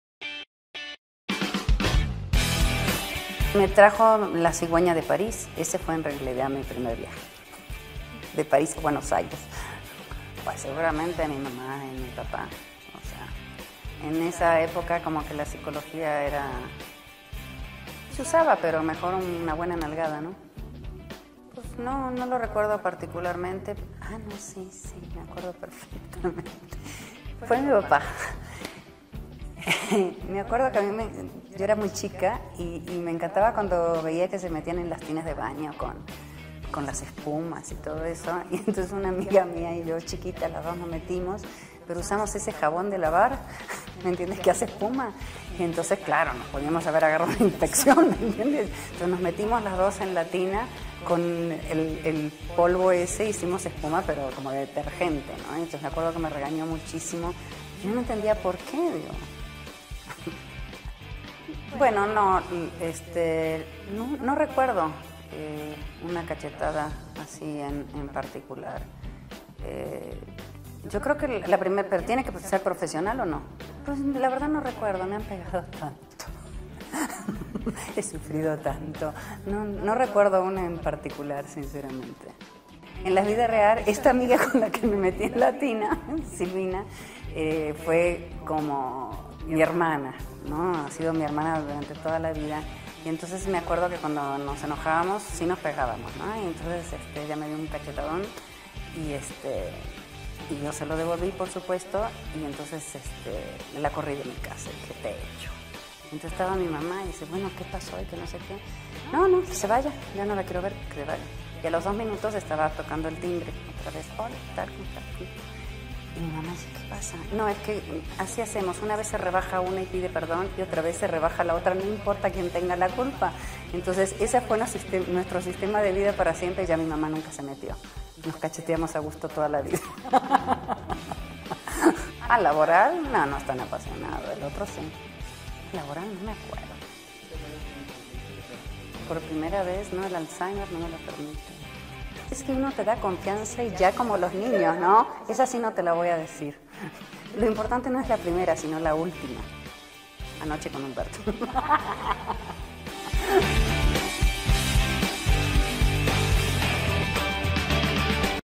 Christian Bach nos cuenta sus primeras veces
La actriz Christian Bach nos cuenta su primer viaje, primer beso, primera escena en la cama, entre otras cosas.